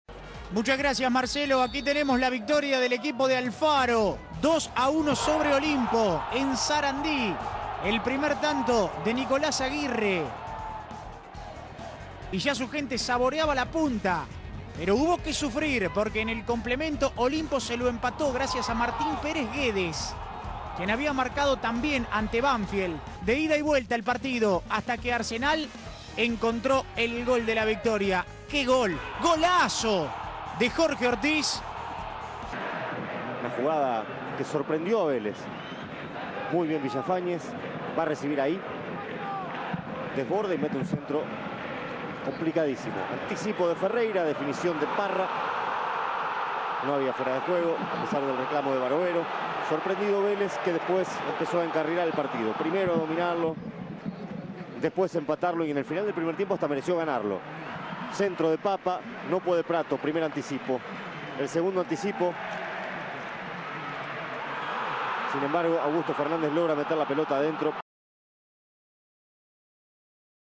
Primera A resumen goles Viernes 6 Fecha 9
Velez 1 - Independiente 1 Arsenal 2 - 1 Olimpo Primera División Fecha 9